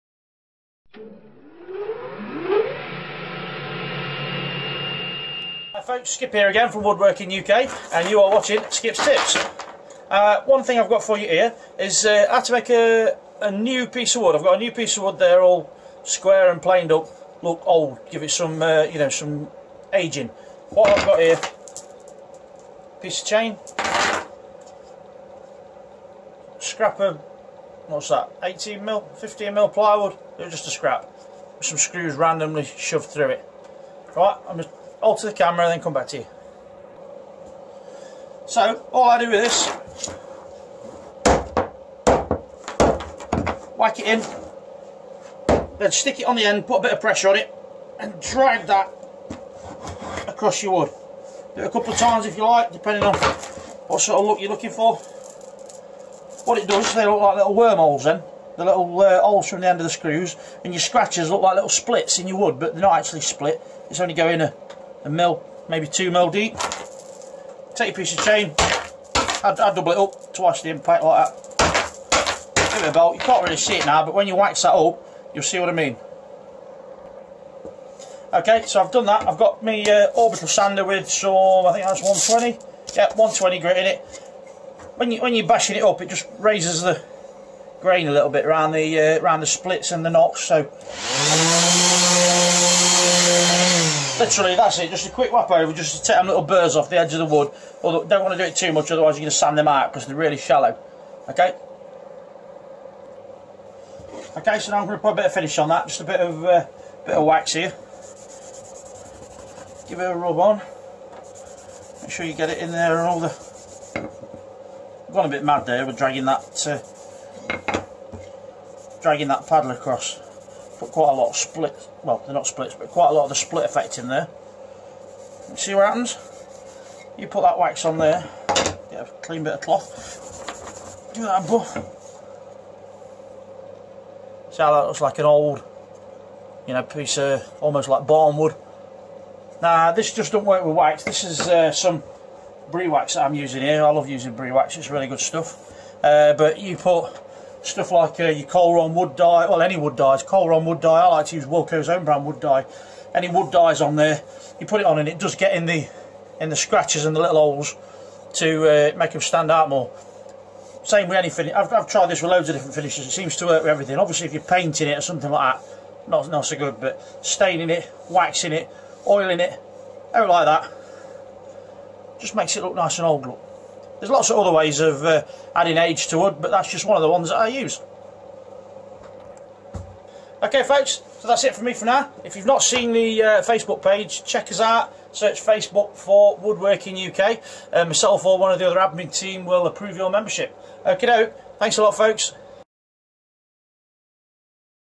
Skip's tips Ageing new wood
How to make a new planes piece of timber have an older barn wood type look
Woodworking UK